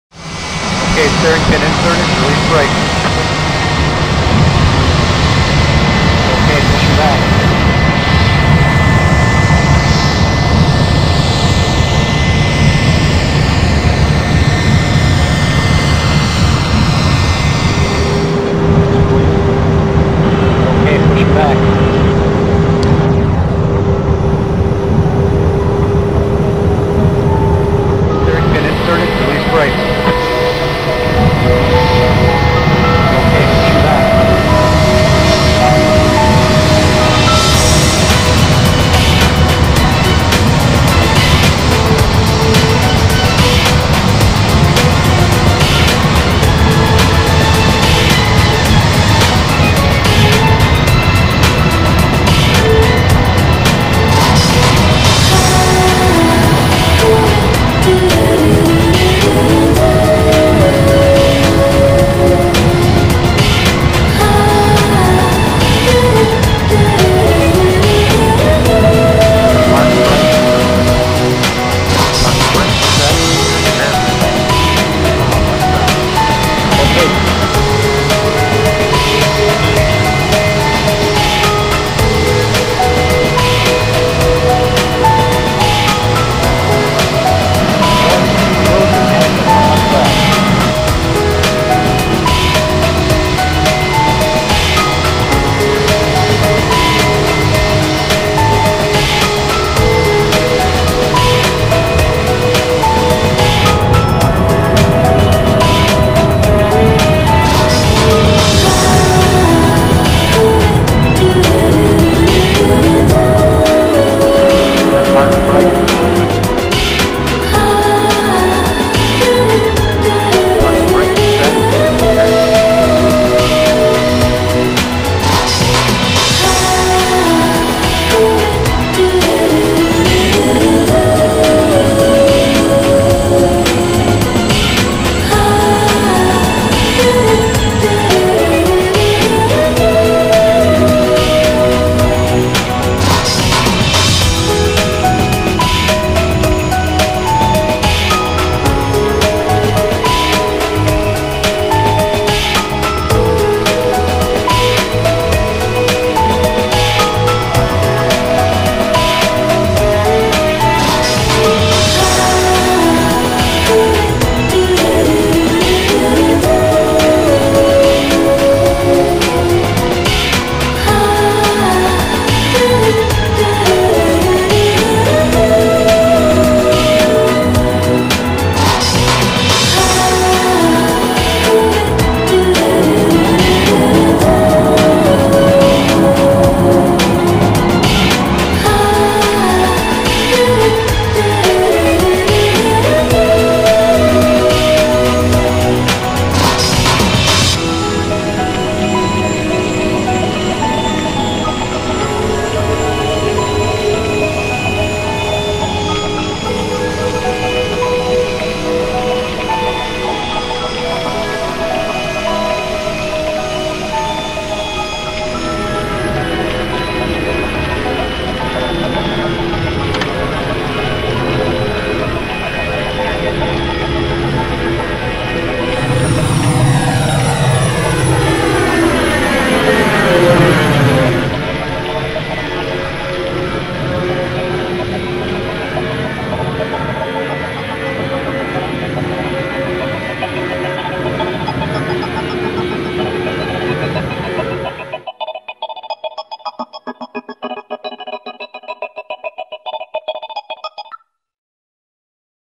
OK Push Back
The aircraft could fly in sky or run forward on ground, but not reverse. Sometimes aircraft should to move backward at the gate or parking area on airport. This backward operation is called Push back. Push back tractor push the aircraft to backward.